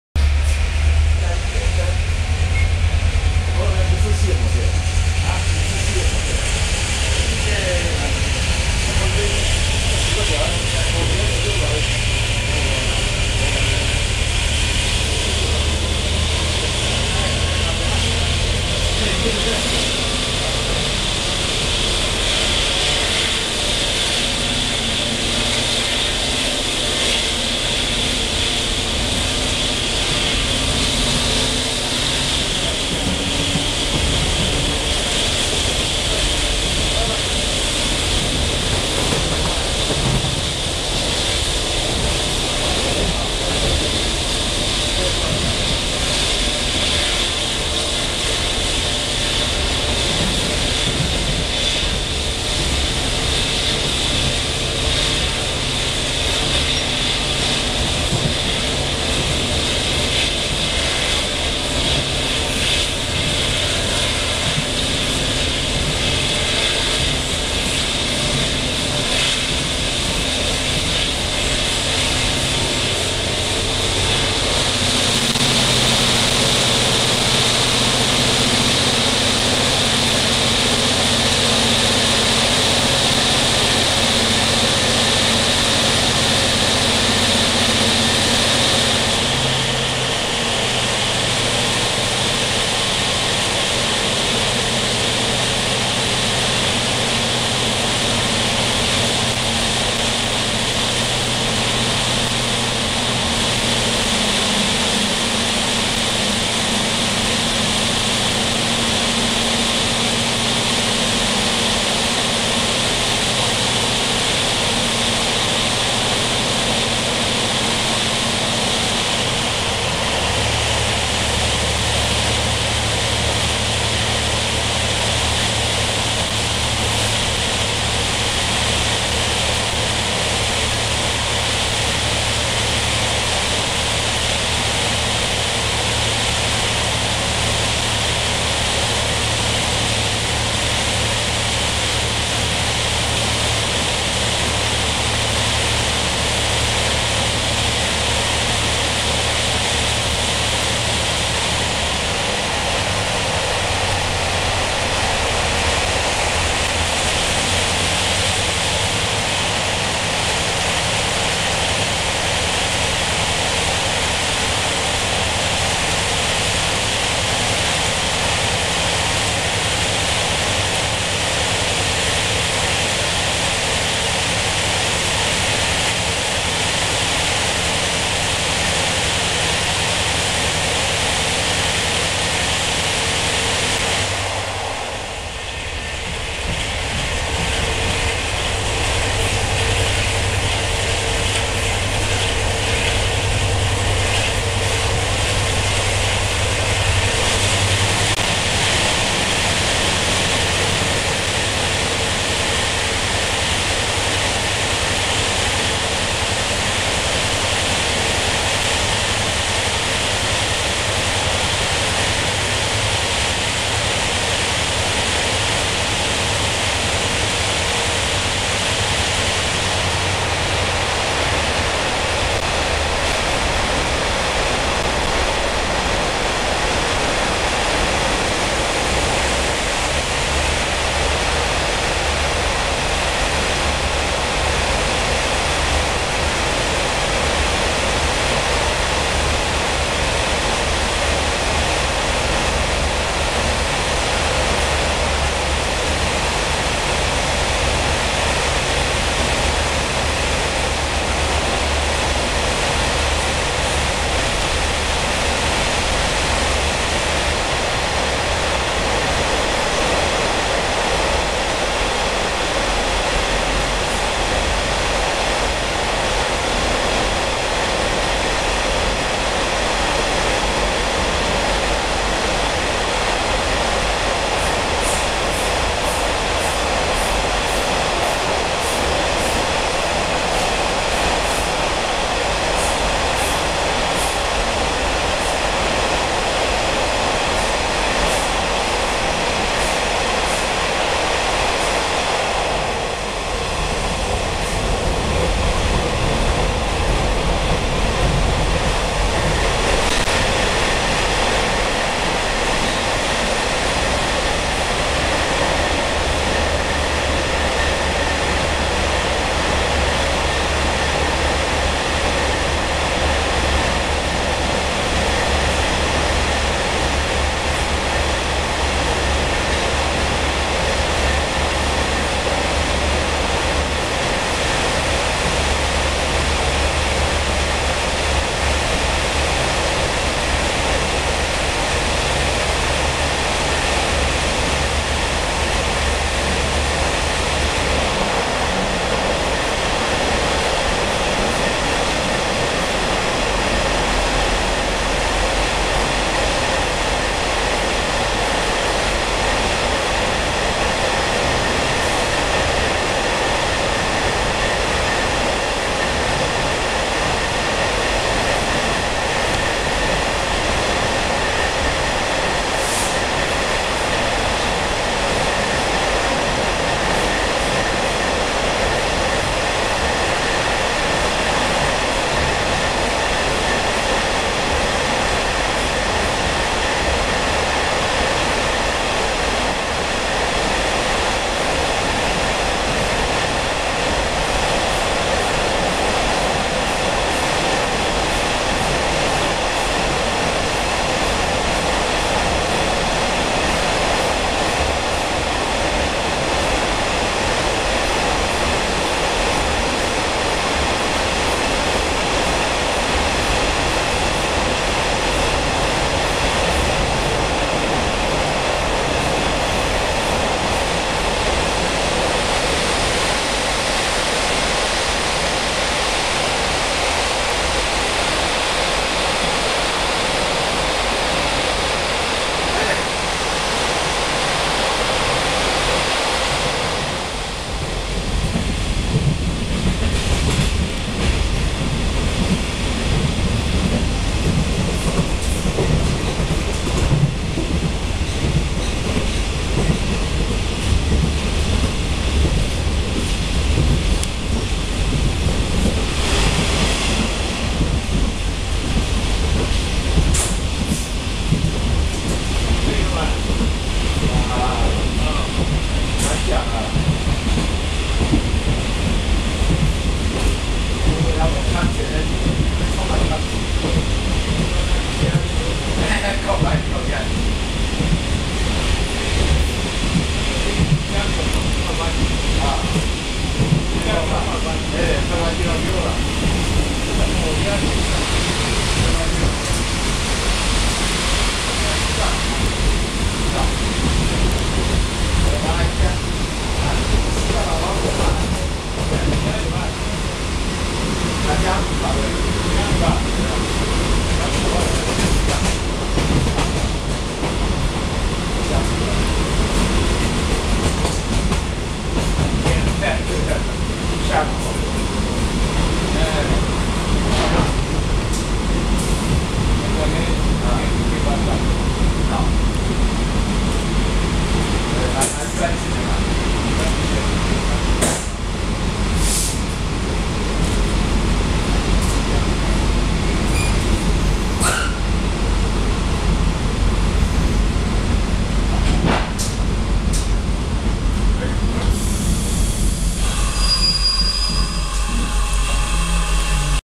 和仁→崇德 533次普快車 車內展望(On Board #533 Train Heren→Chongde, 2010.12.18)
北迴線 和仁→崇德 車內展望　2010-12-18
台鐵 533次普快車(和平往花蓮)。

Taiwan Railway(TRA): On Board #533 Local Train from Heren to Chongde.
Recorded on Dce. 18 2010 by TDCKW.

[HD] 北迴線 和仁→崇德 収録．
台湾鉄路局 普通533号(花蓮行き)

錄影時車廂編號：SPK32732

533次表定時刻：
(以上略)
和仁 08:24開
崇德 08:32到
-(以下略)-

by《鉄道と旅行Augustine's blog》